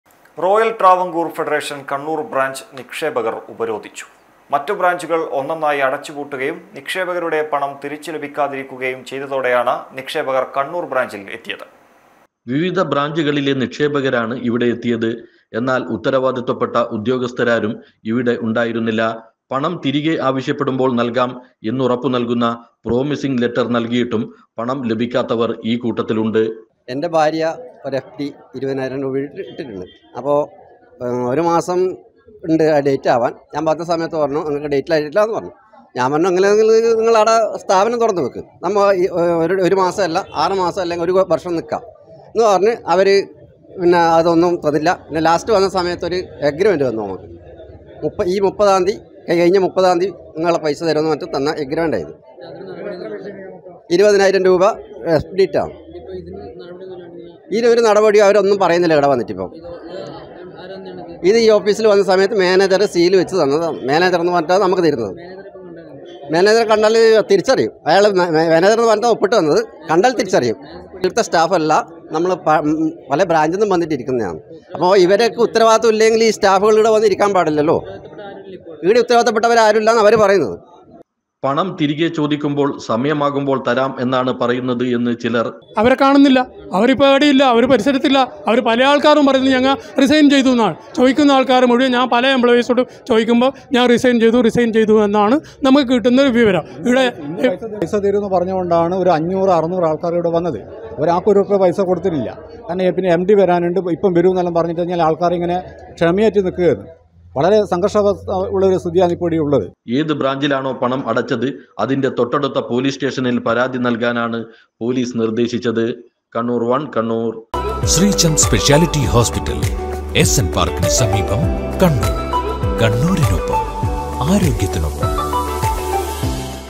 നിക്ഷേപകർ ഉപരോധിച്ചു..
റോയൽ ട്രാവൻകൂർ  ഫെഡറേഷൻ കണ്ണൂർ ബ്രാഞ്ച് - നിക്ഷേപകർ ഉപരോധിച്ചു..